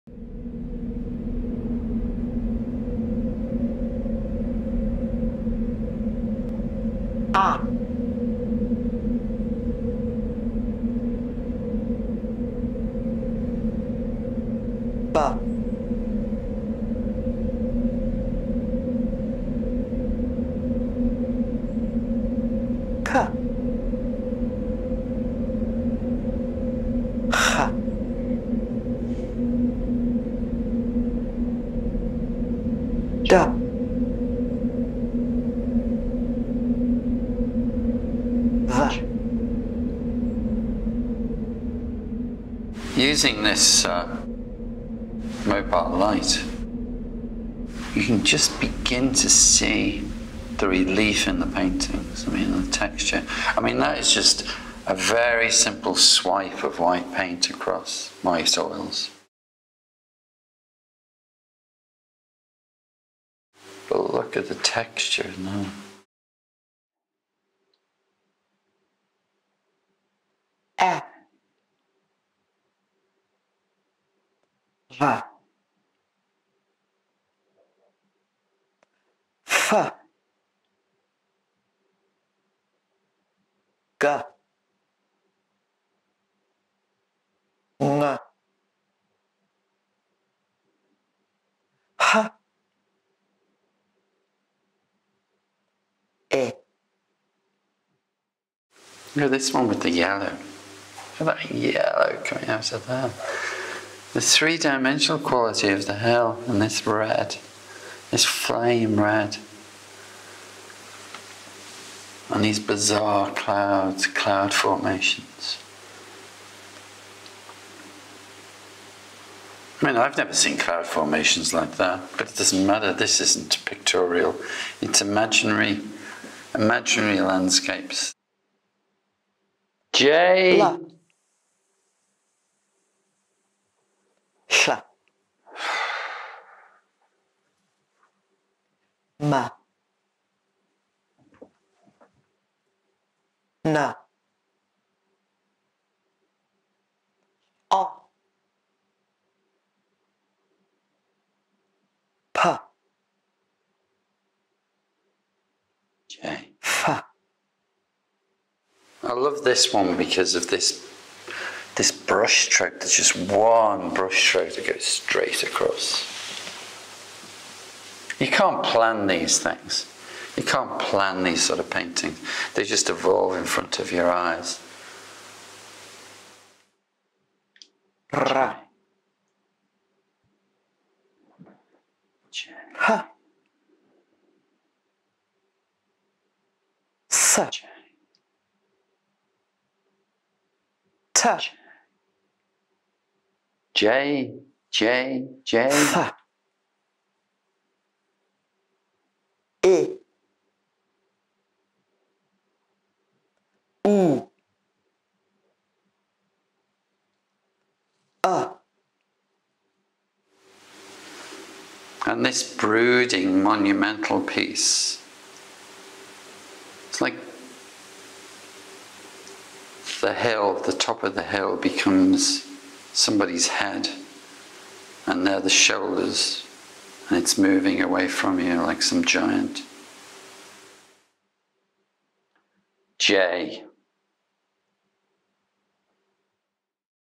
The Vardre in search of Y Faerdre
A 3 minute video promoting an art installation focusing upon Welsh identity through art and language.